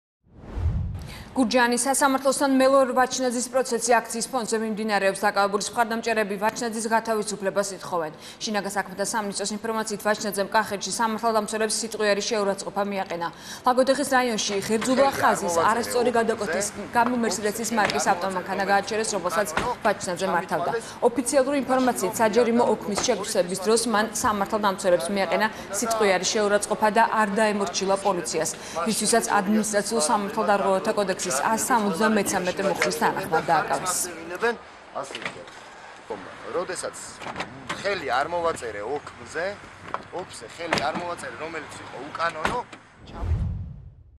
მელორ ვაჩნაძე დაკავებულია
სამართალდამცველებმა პოლიციელებისთვის წინააღმდეგობის გაწევის ბრალდებით გურჯაანში მელორ ვაჩნაძე დააკევეს.